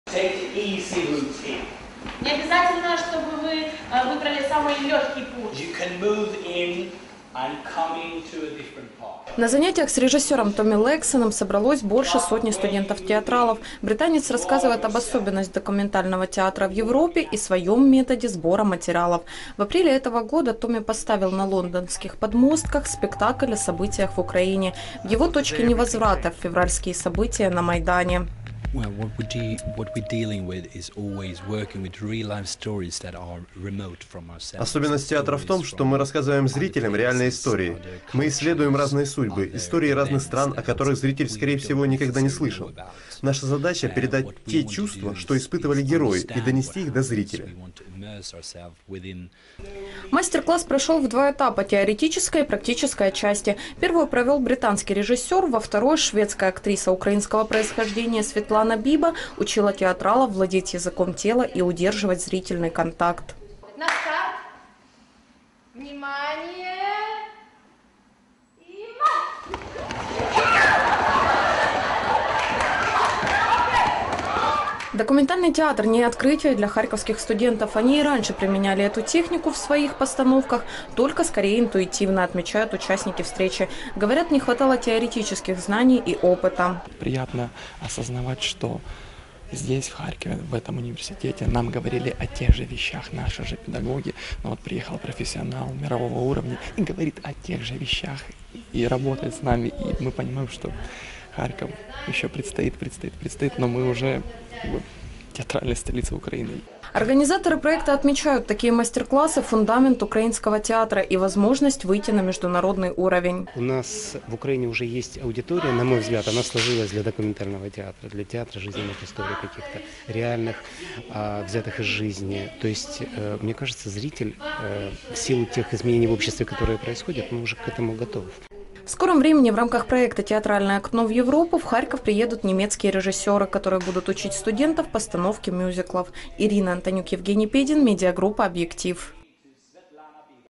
Британский режиссер провел мастер-класс
- Документальное кино на английский лад. Британский режиссер провел мастер-класс для харьковских студентов-театралов - Томми Лексен преподал основы документального театра, когда все постановки на сцене – это реальные истории из жизни. Недавно у себя на родине англичанин поставил знаменитый спектакль «Точка невозврата» о событиях на украинском Евромайдане